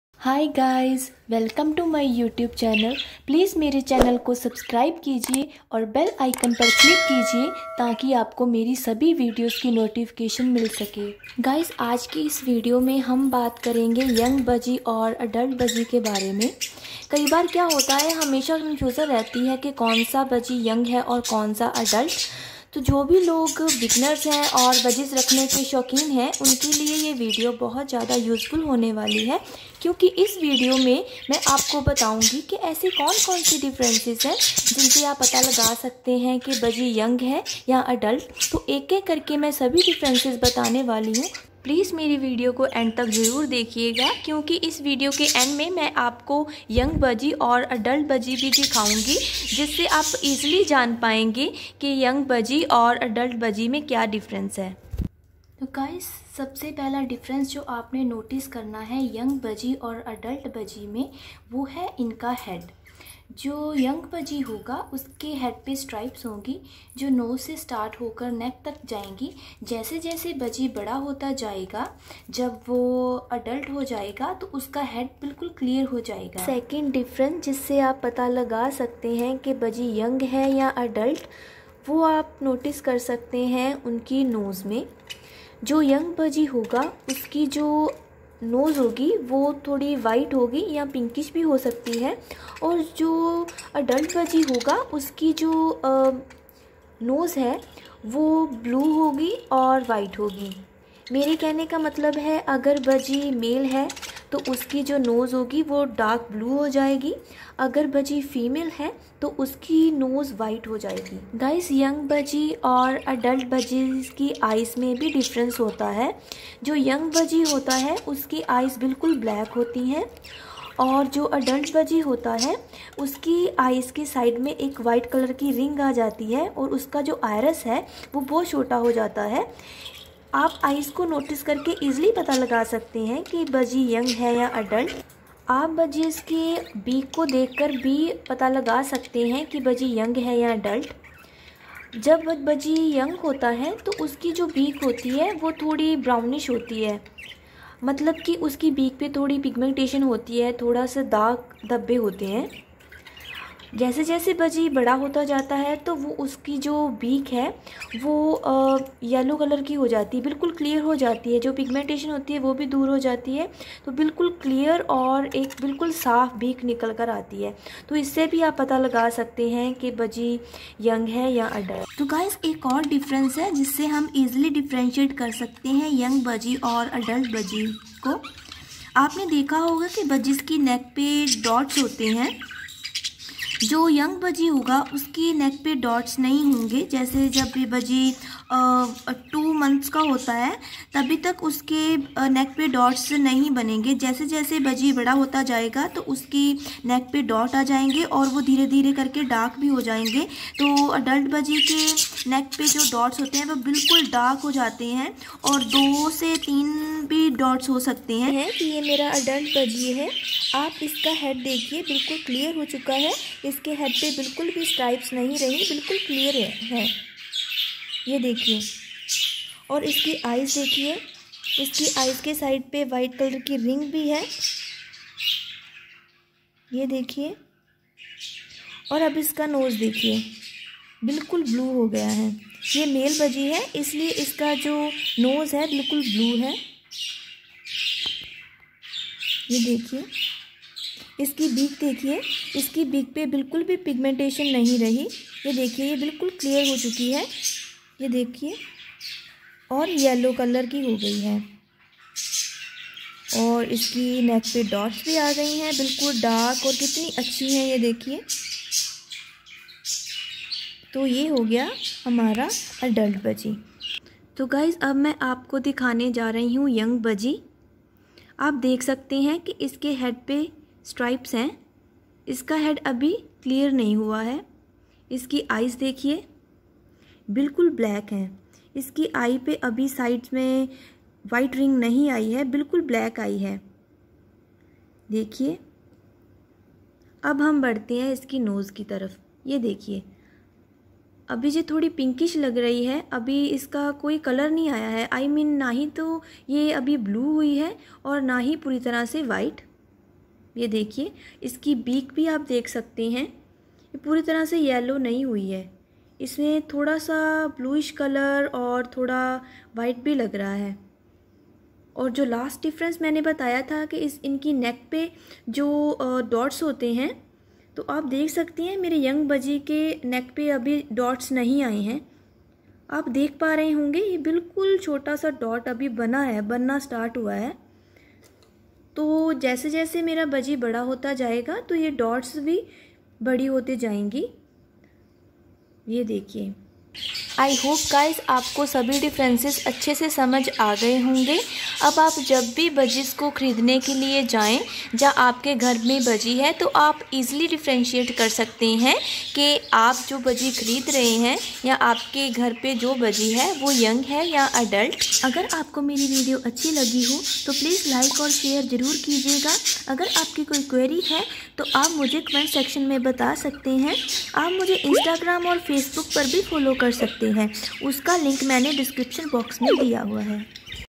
What is difference between Adult budgie and Young budgie | 5 difference | parakeet's identification
five signs in budgies which helps to identify young parakeet and adult parakeet